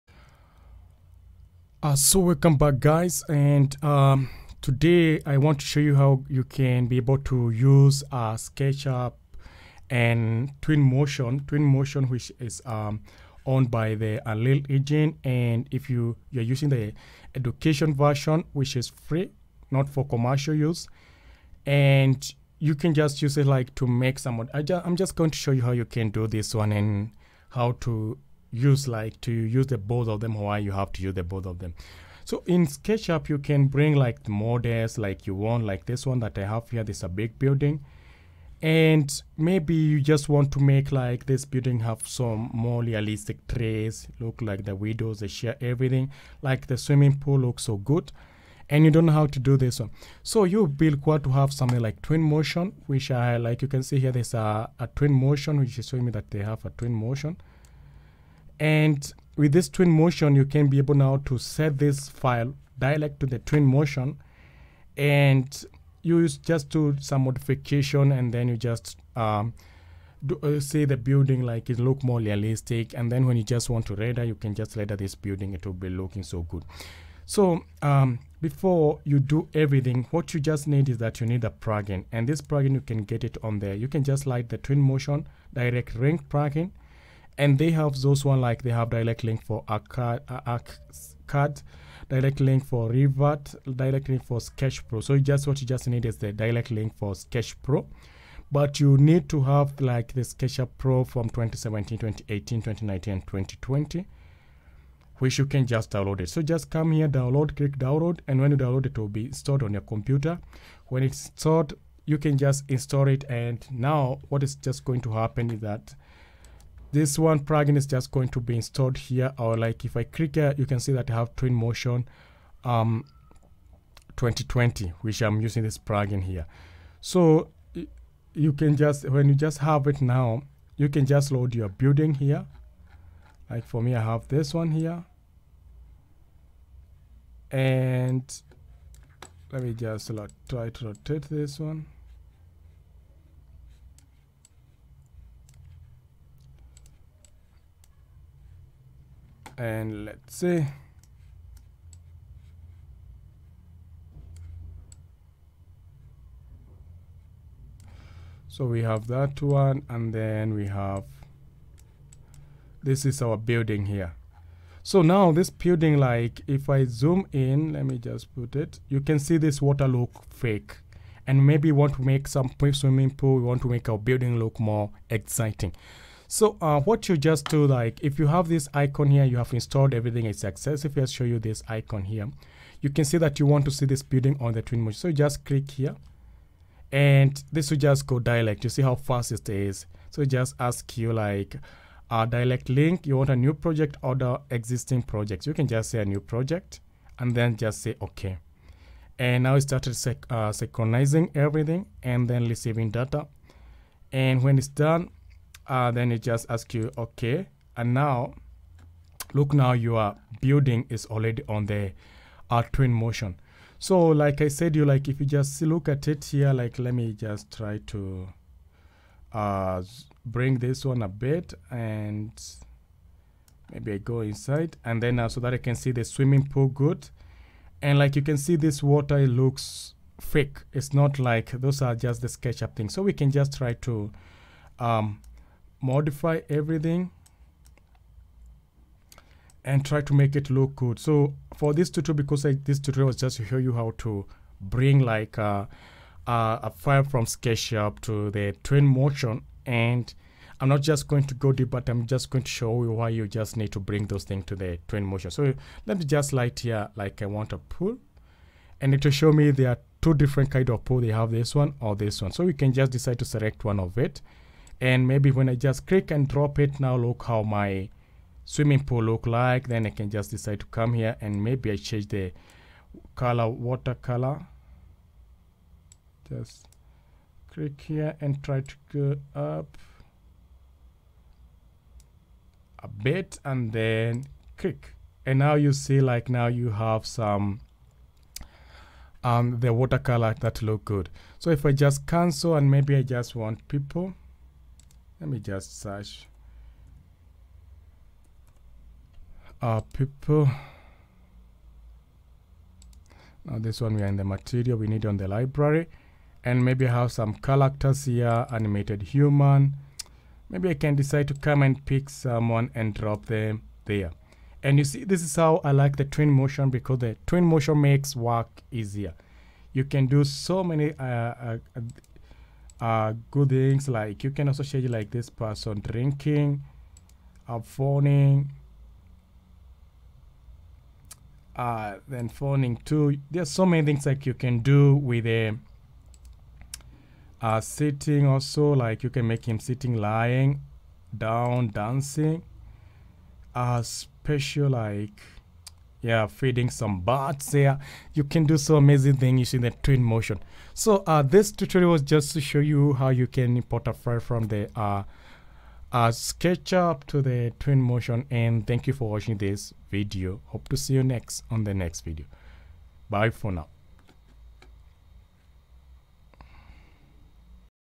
Sketchup - How to Export to Twinmotion Free 3d Models
On this Sketchup tutorial I will show you How to Export to Twinmotion Free 3d Models. this is a good tutorial for ewveryone who want to make amazing and realist 3d models using free twinmotion software.